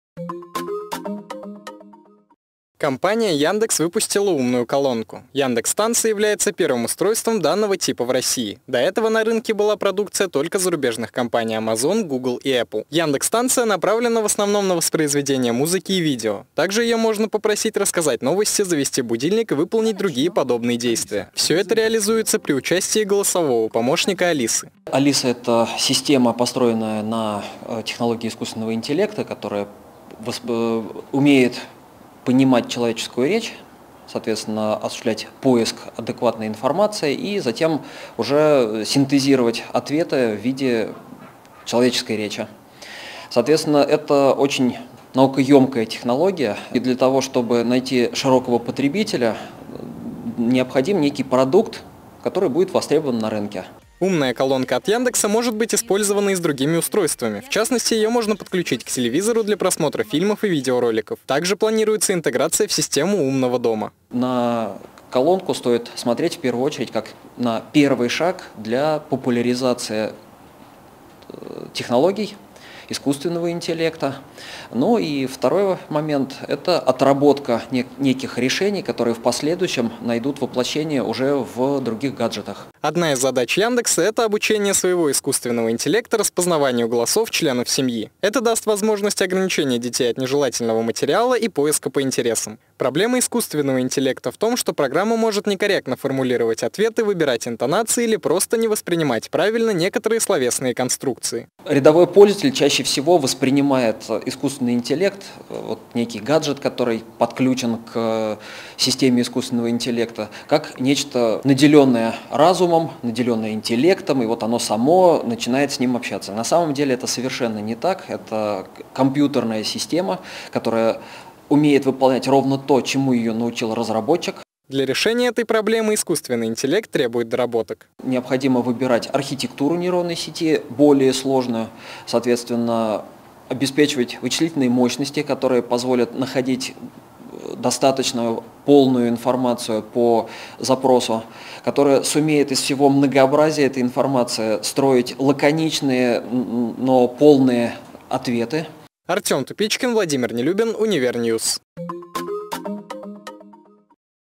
Алиса, включи музыку
На рынке гаджетов появилась «умная» колонка от Яндекса. Что представляет собой устройство и почему за ней стоит будущее